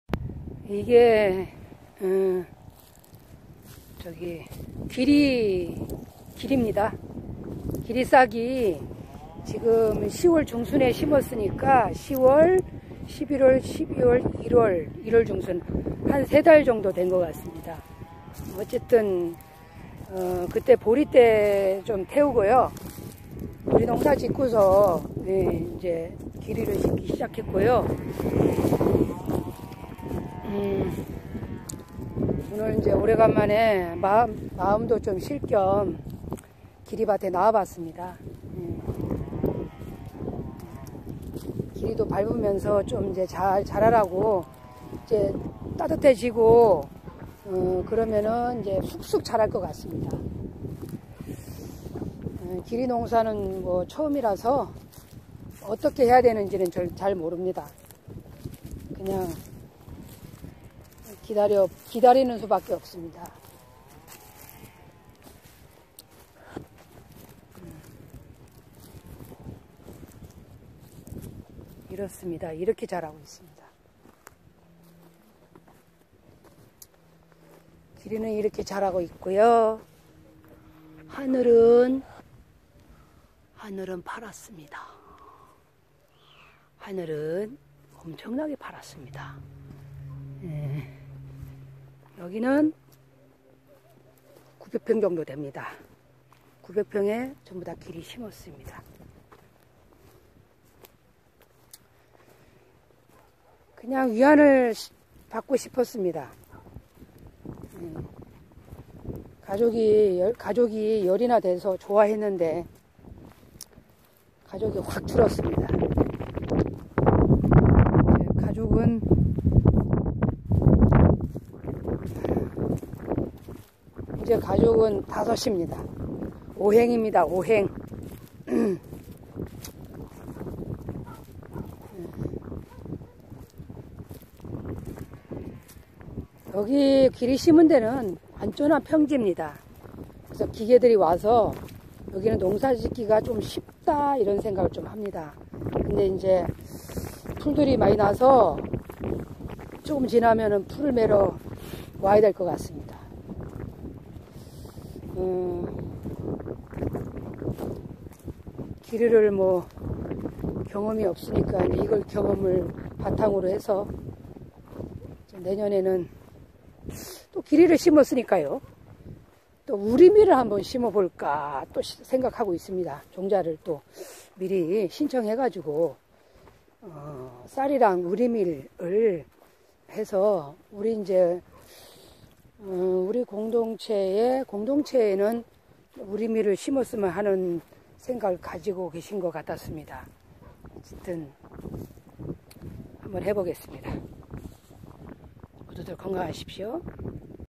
귀리농사일기~2부 작년 10월에 뿌린 귀리 겨울을 견디며 잘 자라고 있습니다.^^
귀리씨  뿌리고  잊고  살았다. 세달이  흘르고  귀리는 깊게  뿌리를  내리고  있다.